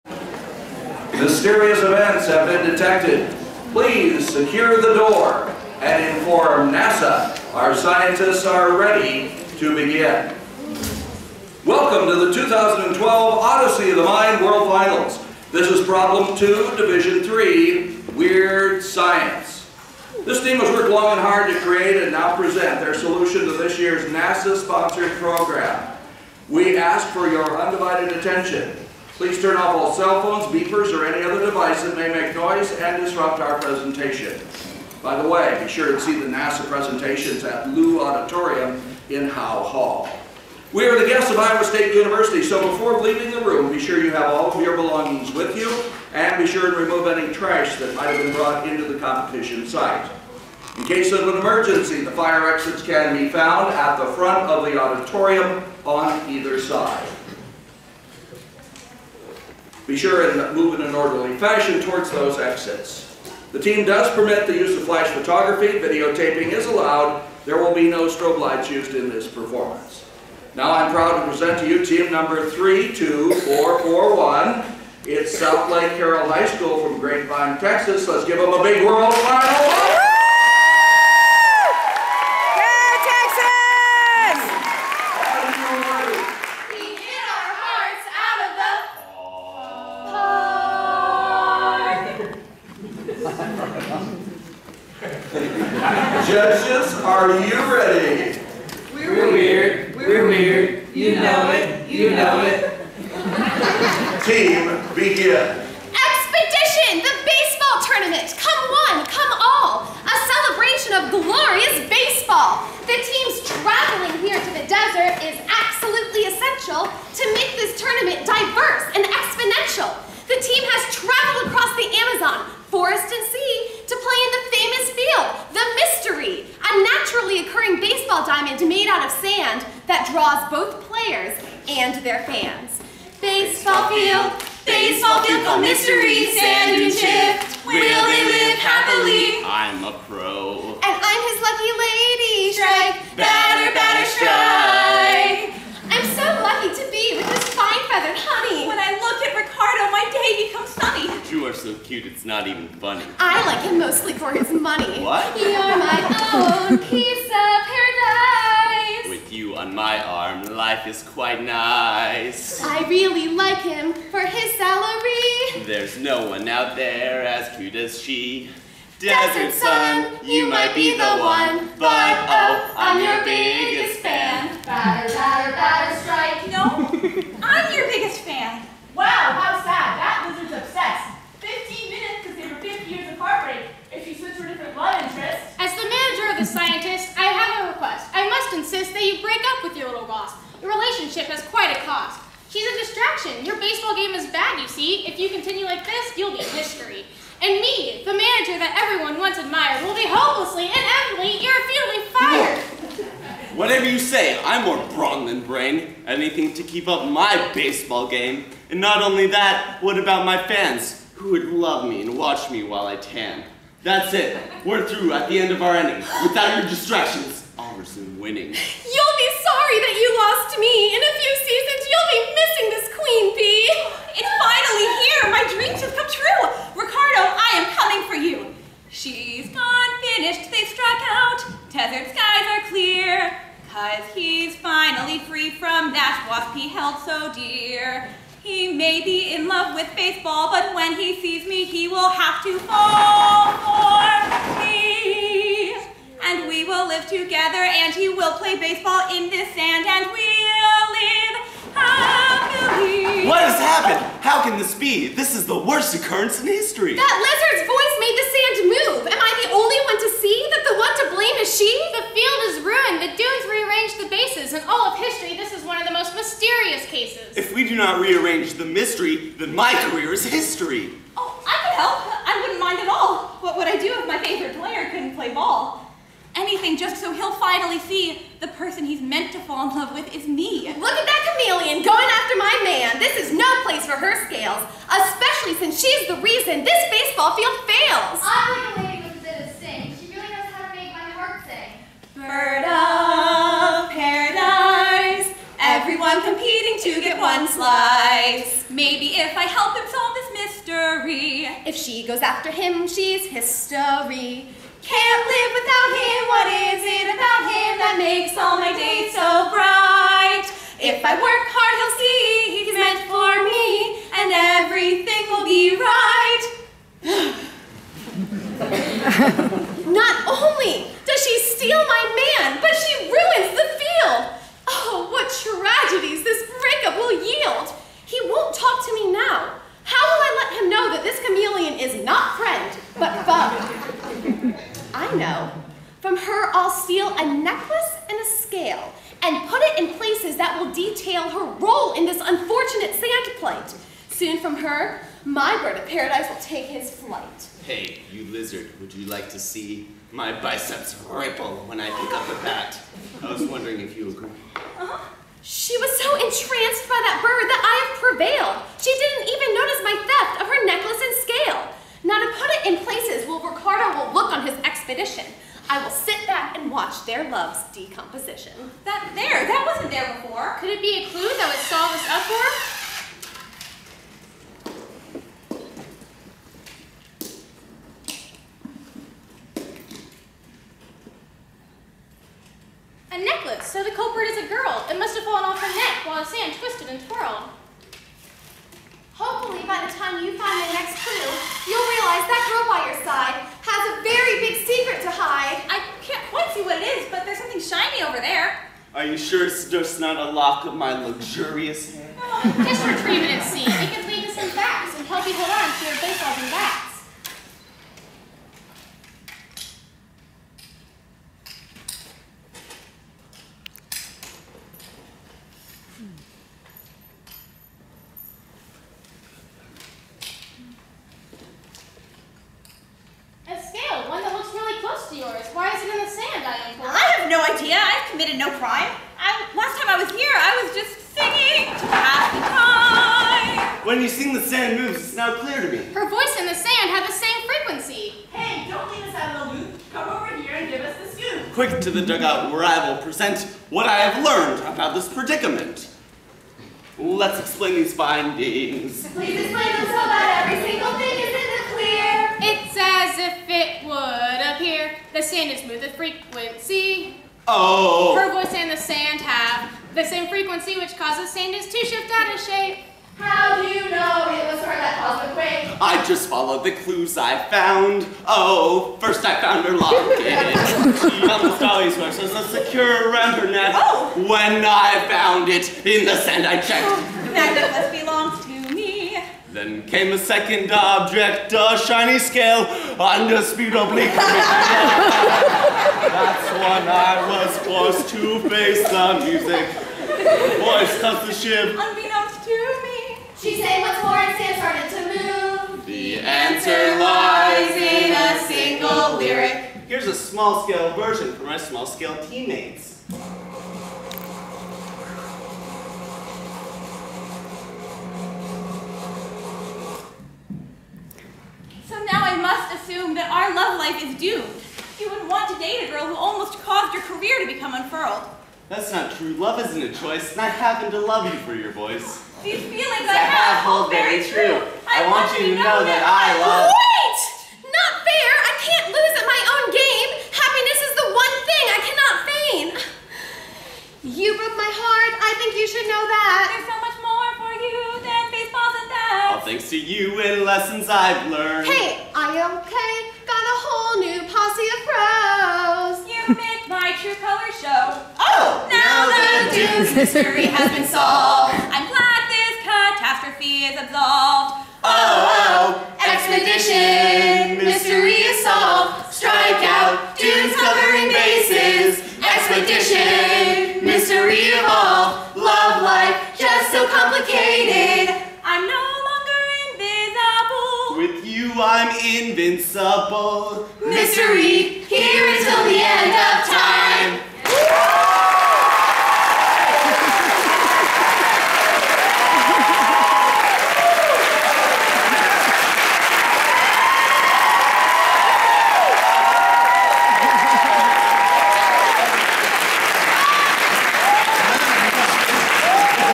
2012 Odyssey of the Mind World Finals -Carroll High School Technical 2nd Place Winners!
Odyssey of the Mind World Finals 2012
Carroll High School Technical Team
2nd Place Division 3
Weird Science